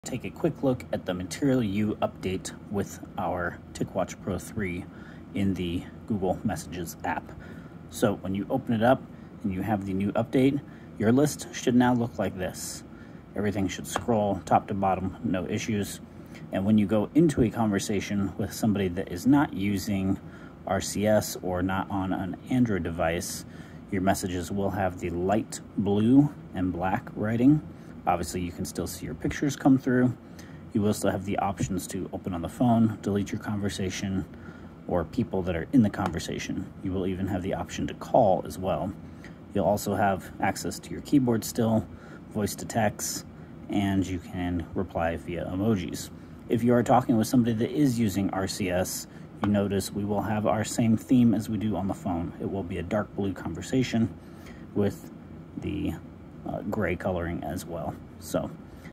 Wear OS/Material You/ Google Messages
I've been using the NEW messages by Google with the Material U overhaul on my Tic watch pro 3 since it was available. Its WAY Faster then it use to be.